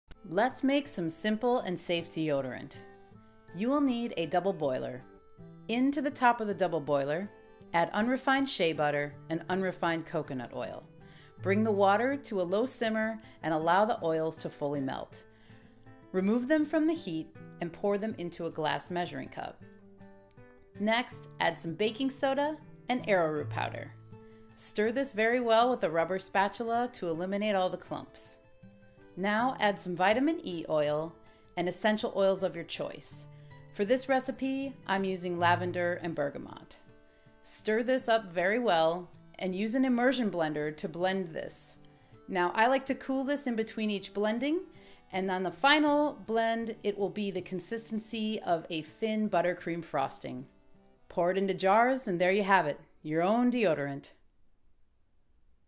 How to Make Simple & Safe Deodorant
Are you a deodorant person? Not everyone is. If you happen to be, here's a short tutorial for you on how I like to make deodorant, Enjoy!